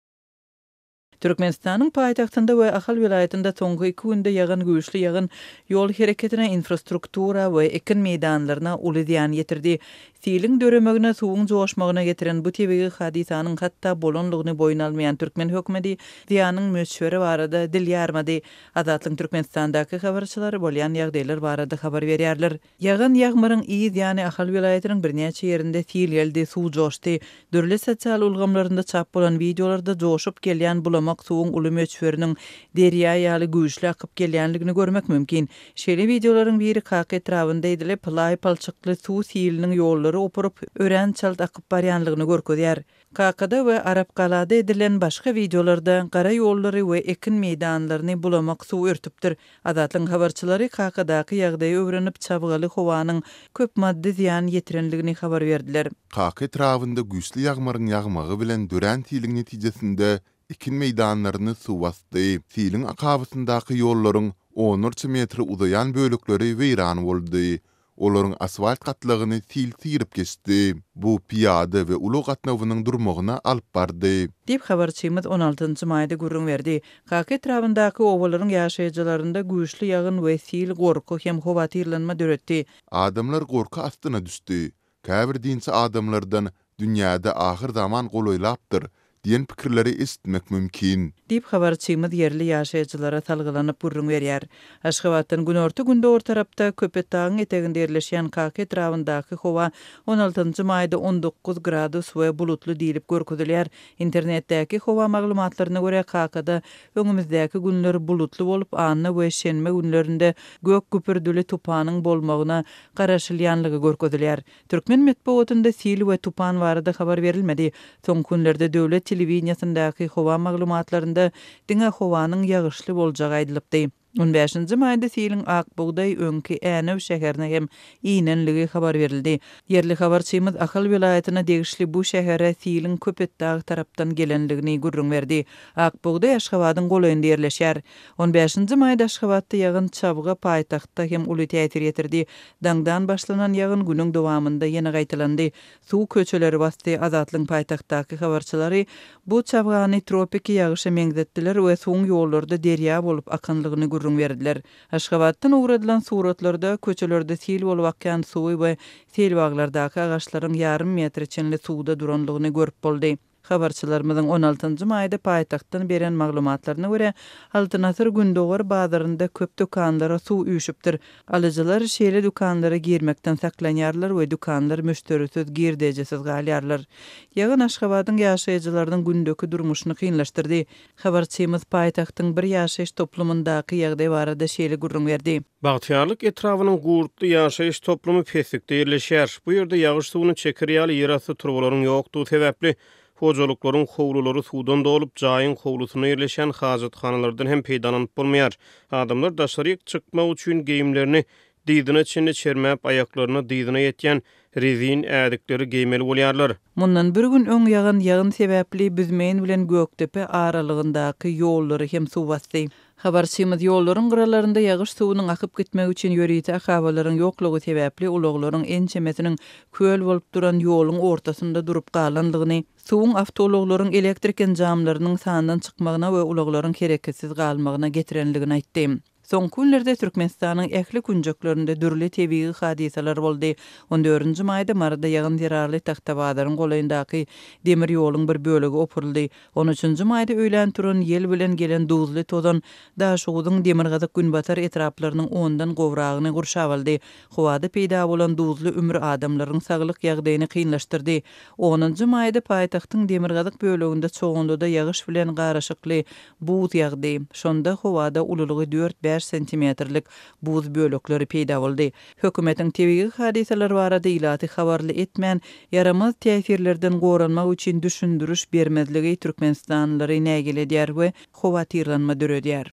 Ahal welaýatynda we paýtagtda ýagyş uly zyýan ýetirdi
Türkmenistanyň paýtagtynda we Ahal welaýatynda soňky iki günde ýagan güýçli ýagyn ýol hereketine, infrastruktura we ekin meýdanlaryna uly zyýan ýetirdi. Siliň döremegine, suwuň joşmagyna getiren bu tebigy hadysanyň hatda bolandygyny boýun almaýan hökümet zyýanyň möçberi barada dil ýarmaýar. Azatlygyň Türkmenistadaky habarçylary bolýan ýagdaýlar barada habar berýärler.